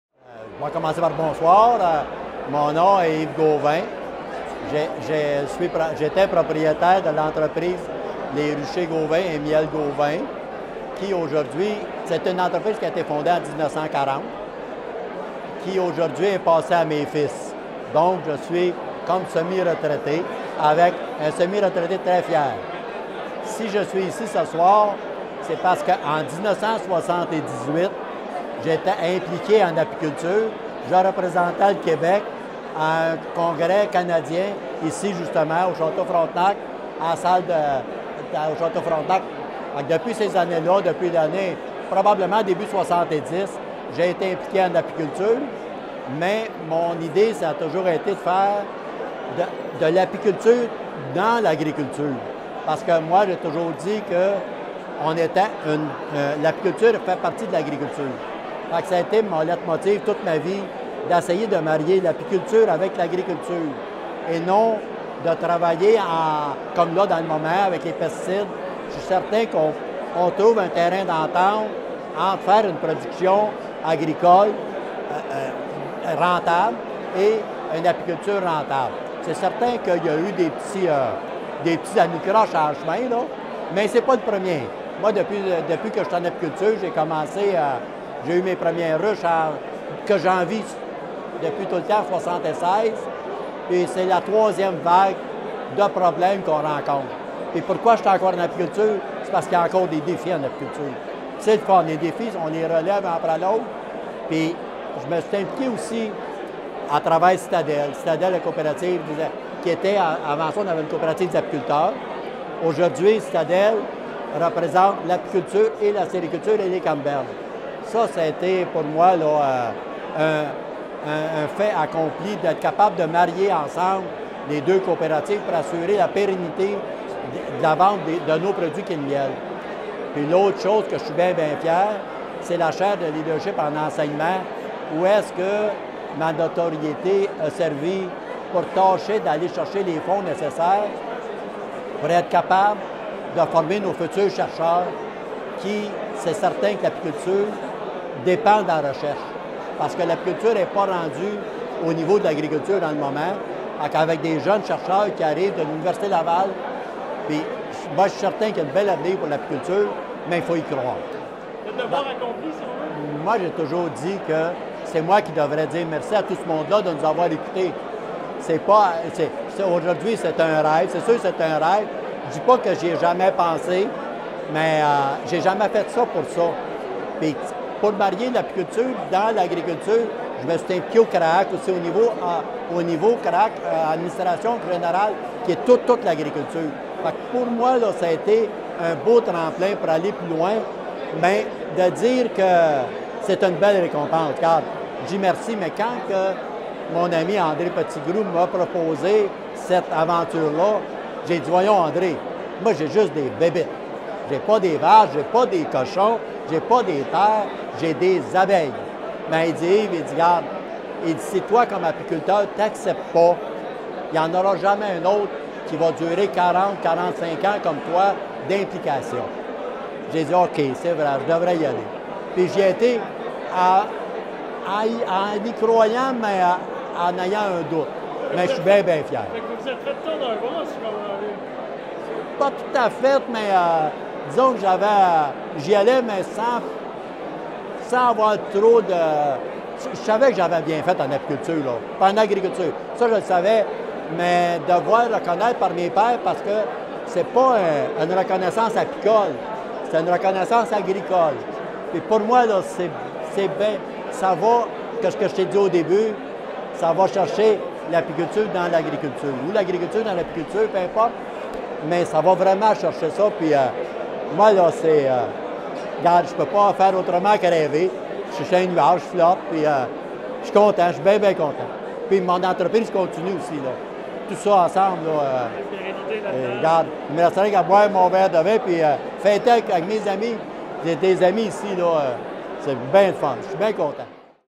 Réaction d'Yves Gauvin à son intronisation
M. Yves Gauvin, apiculteur, réagit à son intronisation au Temple de la renommée de l'agriculture du Québec, lors du gala qui a eu lieu le 28 septembre 2019, au Château Frontenac, à Québec.